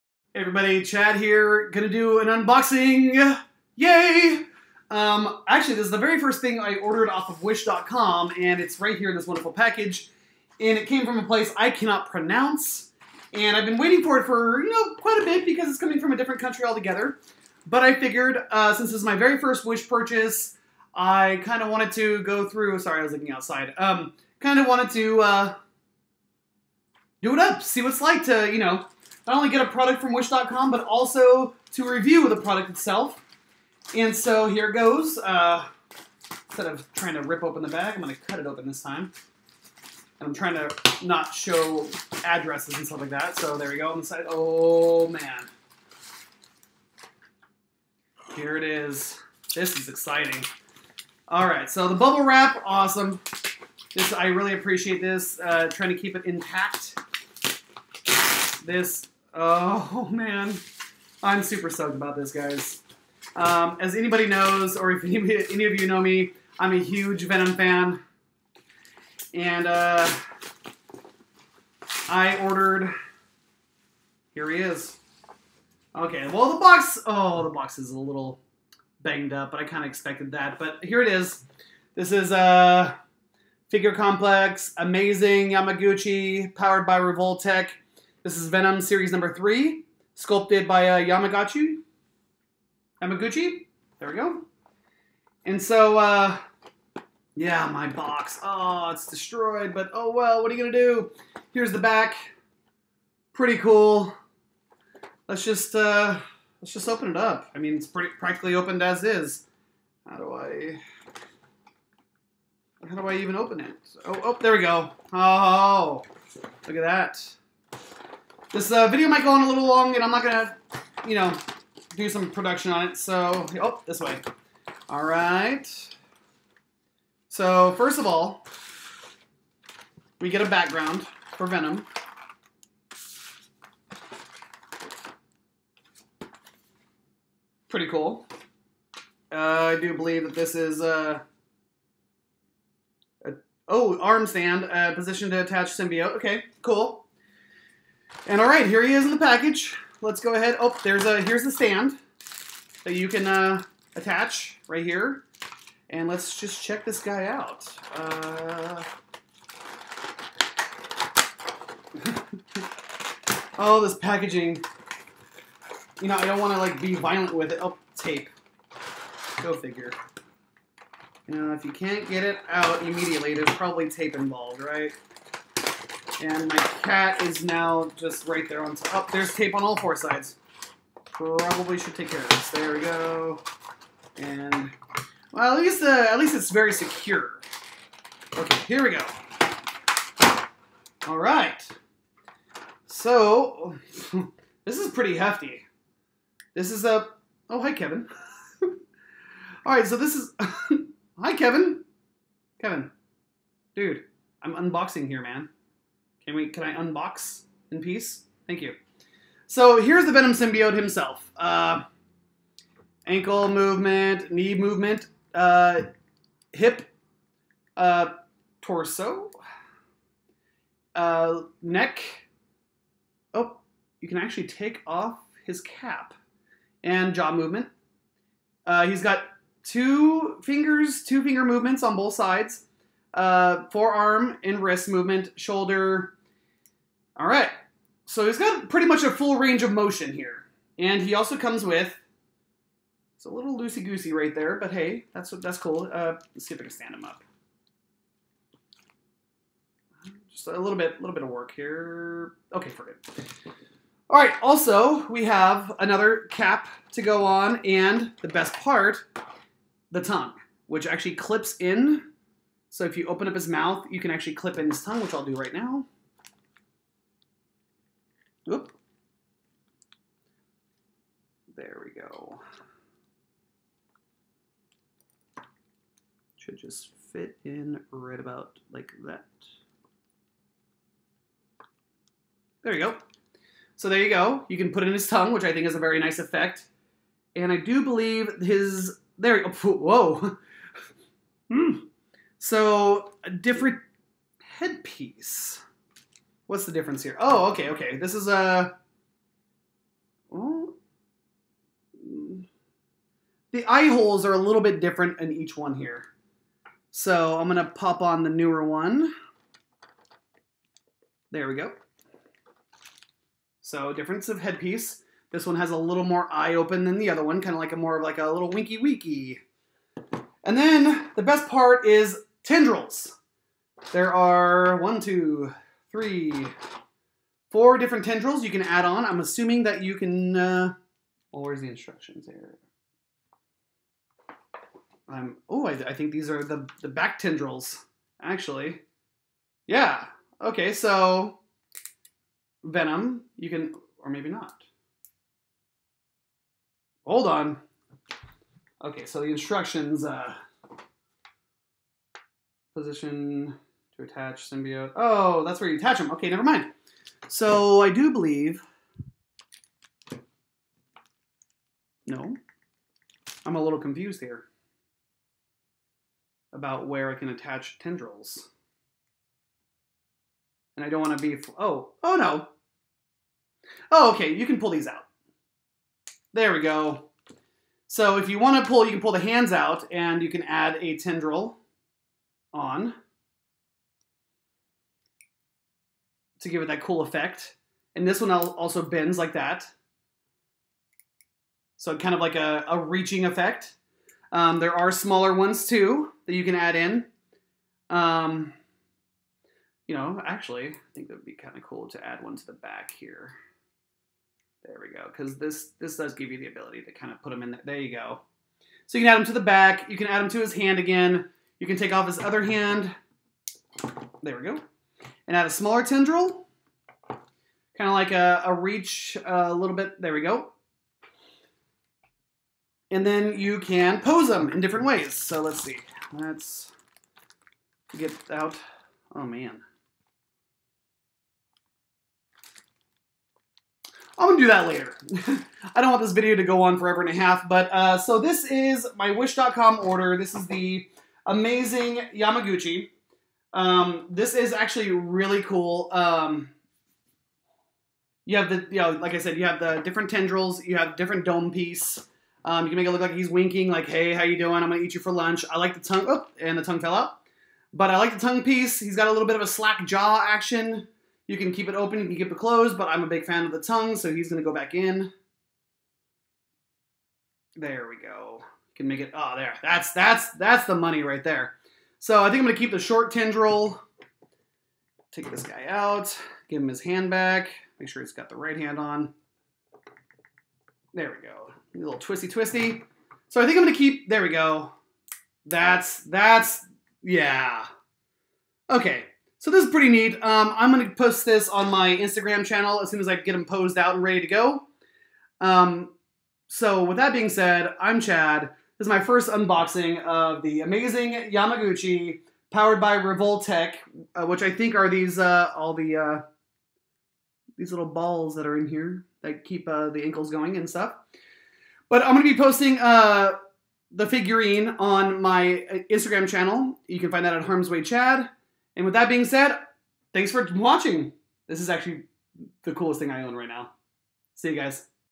Unboxing - Amazing Yamaguchi Venom
My first Wish.com order came in and did it live up to my expectations? Unboxing of the Amazing Yamaguchi Venom Series 3